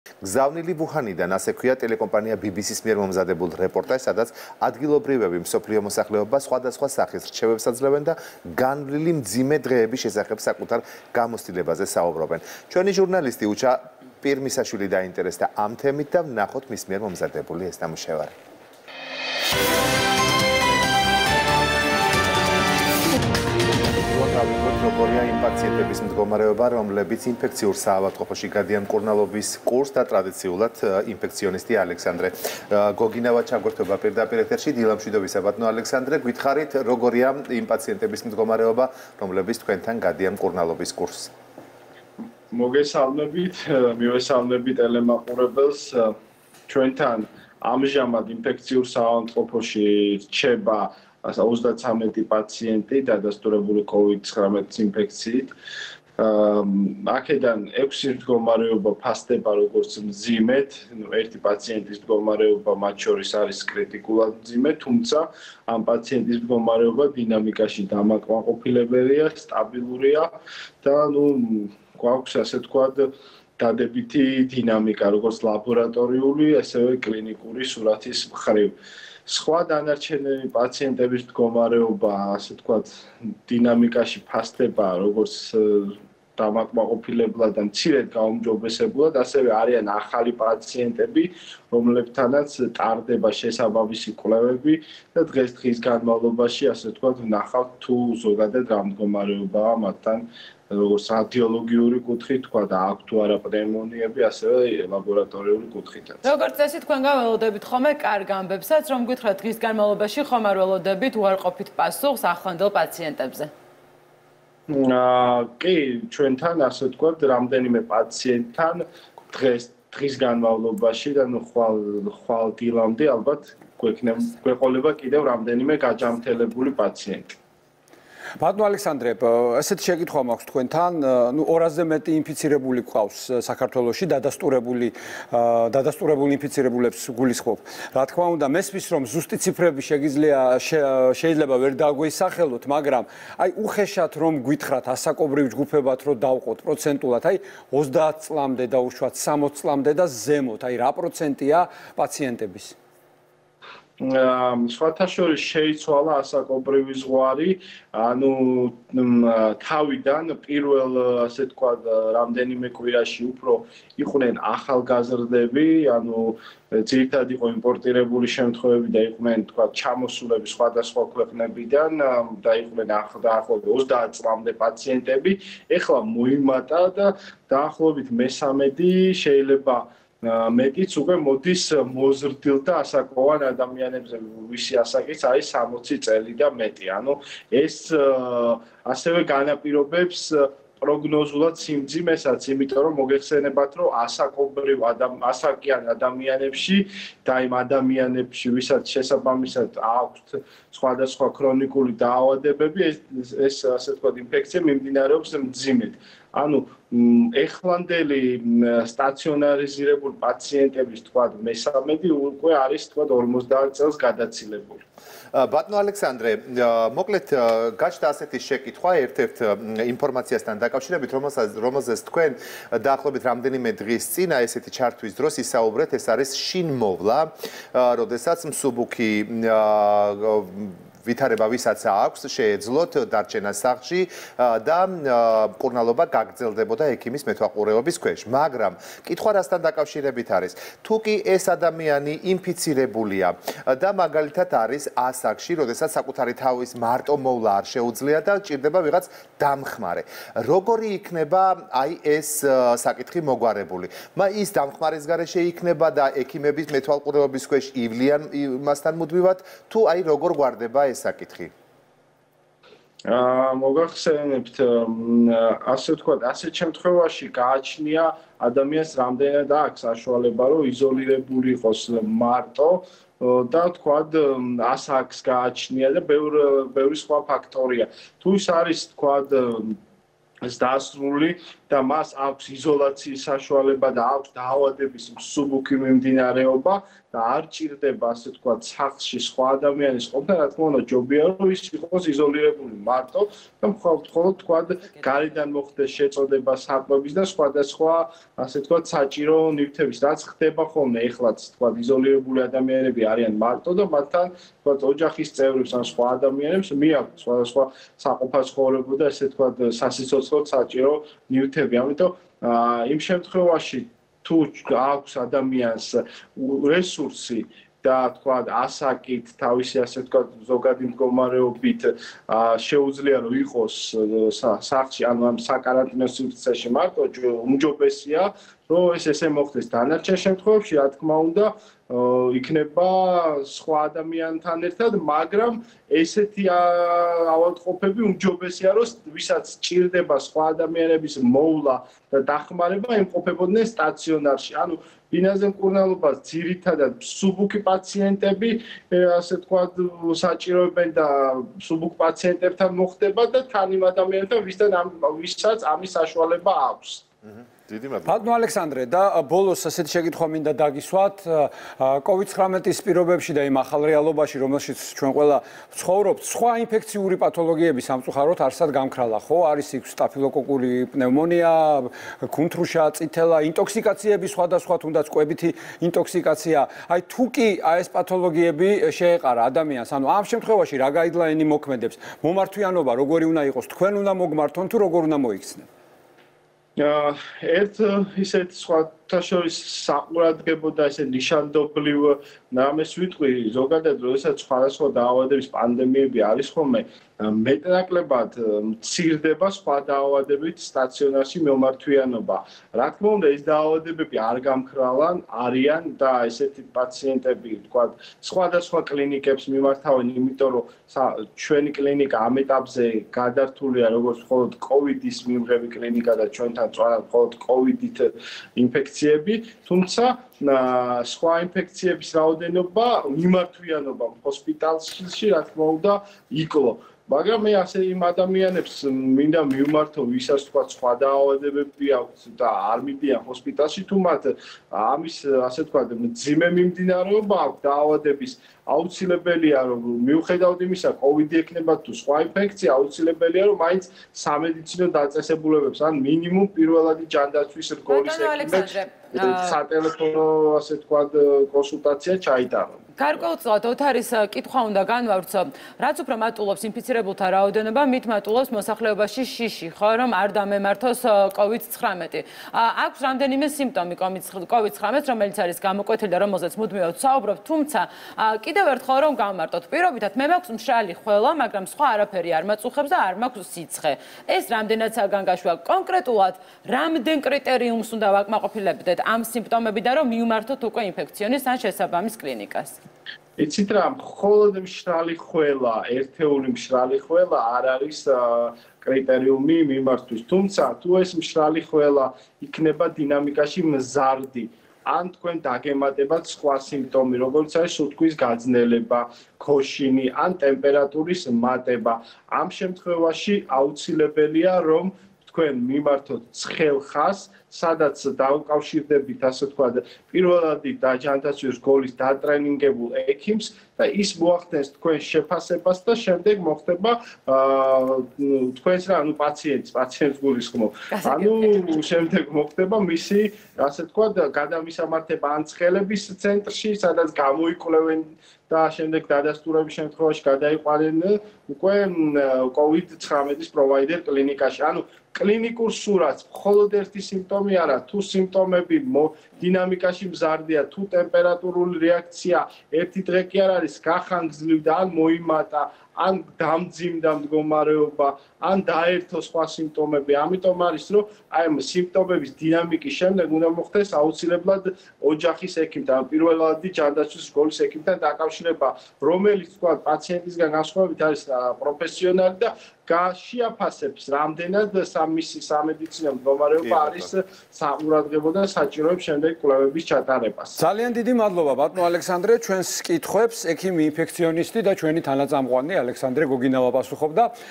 ვითარება ინფექციურ საავადმყოფოში - 10 აპრილი
პირდაპირიჩართვა ინფექციონისტ ალექსანდრე გოგინავასთან